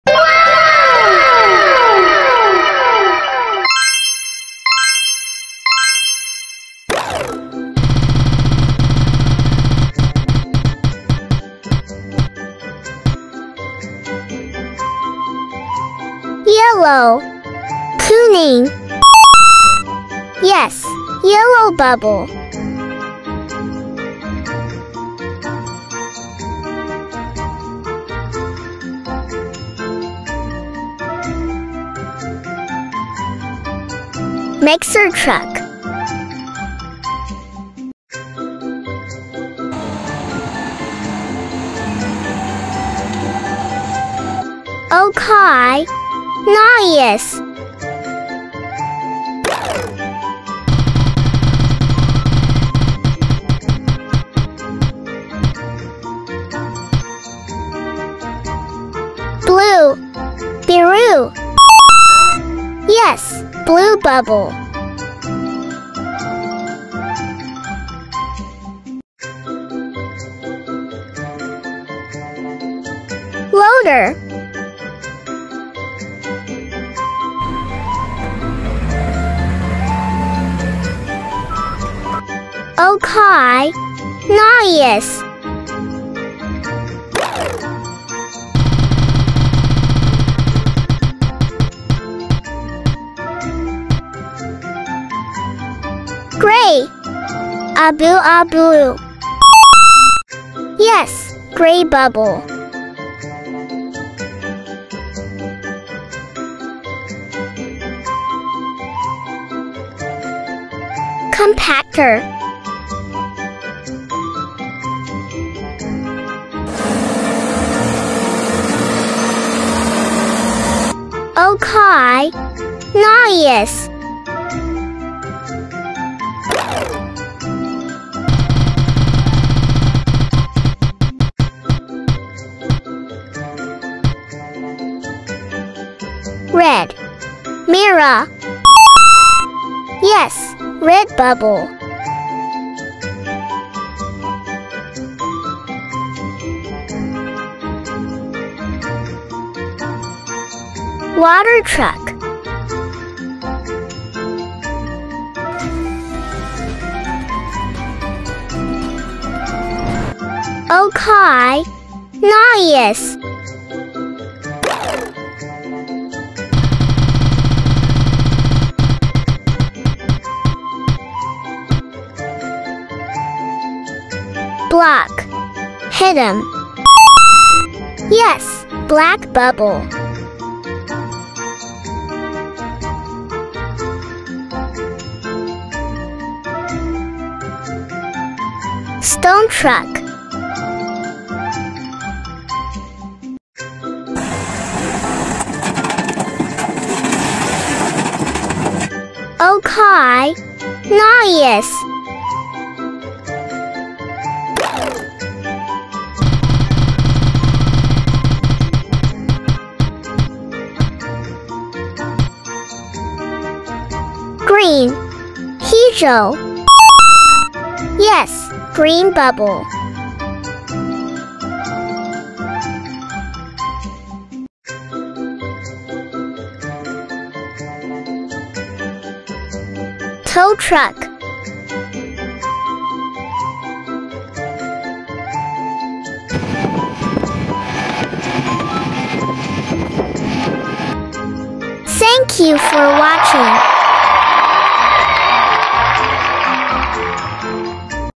Loader VS Compactor VS Stone Truck VS Mixer Truck VS Water Truck VS Tow Truck
Mari mengenal macam - macam TRUK KONTRUKSI TERPOPULER

Loader
Silinder
Truk Batu
Truk Molen
Truk Tangki Air
Truk Derek